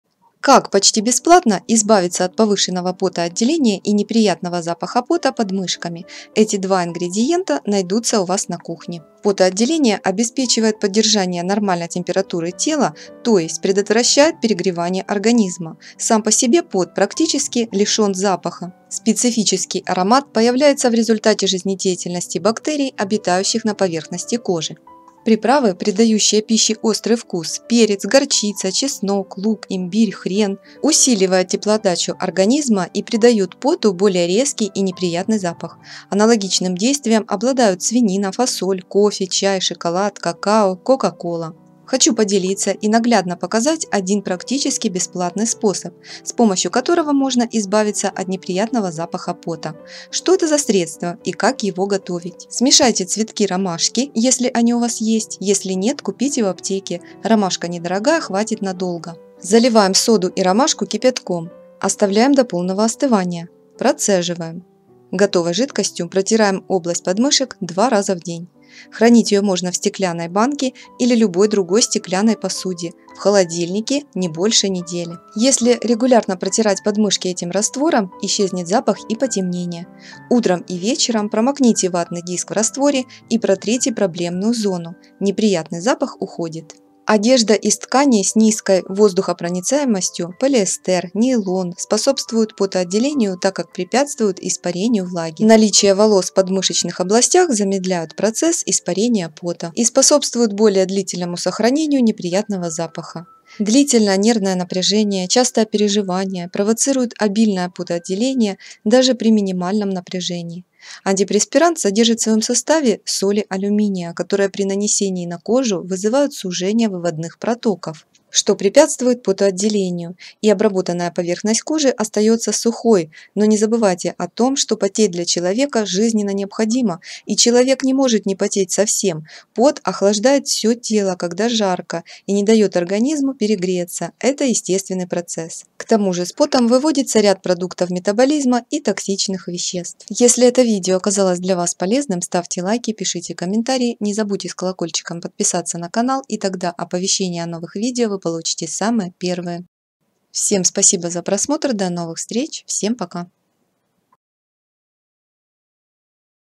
КАК избавиться от неприятного запаха ПОТА под мышками, простым домашним средством.
Неприятный аромат появляется в результате жизнедеятельности бактерий обитающих на поверхности кожи.

столовая ложка цветков ромашки
пол чайной ложки соды
пол литра воды